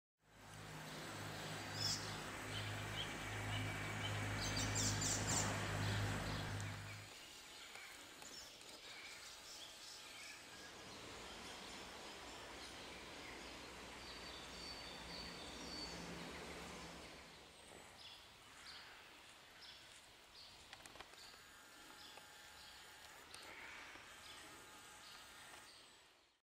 Mhorrgazellen-Nachwuchs jetzt auf der Außenanlage im Tierpark Berlin
Mhorrgazellen-Nachwuchs jetzt auf der Außenanlage im Tierpark Berlin: Im Tierpark Berlin gab es dreifachen Nachwuchs bei den Mhorrgazellen, die am 21.04.22 und 25.04.22 geboren sind. Sie waren die letzten Tage schon im Vorgehege hinter Gittern zu sehen. Ab sofort kann man die Drei mit ihrer Familie auf der Außenanlage beobachten.
Ihren Namen verdanken die tagaktiven Mhorrgazellen ihrer Färbung - "mhorr" bedeutet auf Arabisch "Fohlen"-, da sie mit ihrer dunkelbraunen Rückenfärbung den Fohlen der arabischen Pferde ähnlich sehen. Sie leben in kleinen Gruppen. Angeführt wird eine gemischt-geschlechtliche Herde von einem dominanten Männchen.